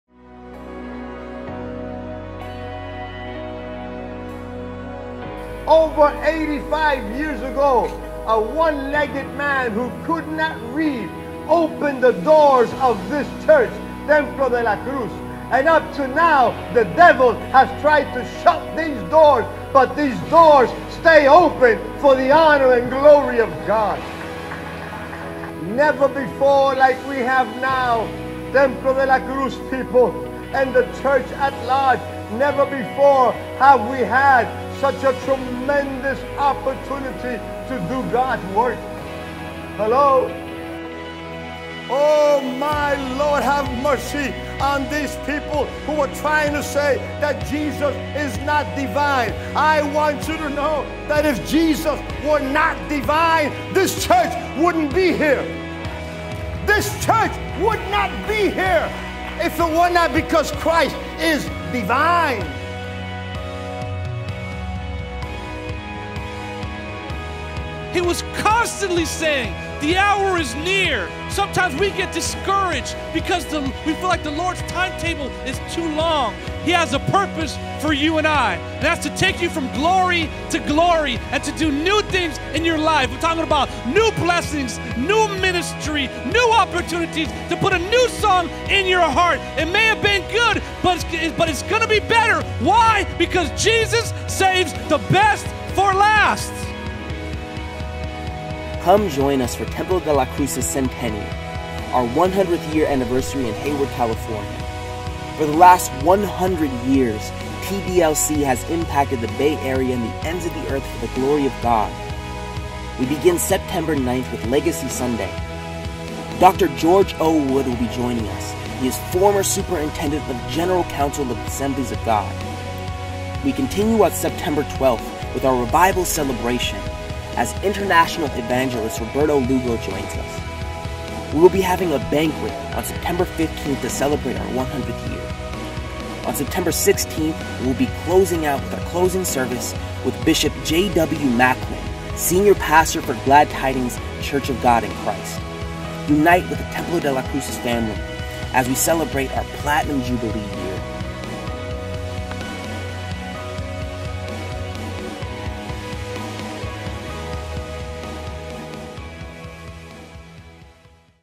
TDLC 100th Anniversary Celebration
The Year of Jubilee is here!

Come join Templo De La Cruz for it's 100th Anniversary Jubilee Celebration!

Schedule:

Sept. 9 @ 10am - Legacy Sunday

Sept. 12-14 @ 7pm - Revival Celebration

Sept. 15 @ 4pm - Platinum Jubilee Banquet*

Sept. 16 @ 10am - Closing Sunday

* For tickets search on Eventbrite: TDLC Jubilee Banquet

Address:

Templo De La Cruz
24362 Thomas Ave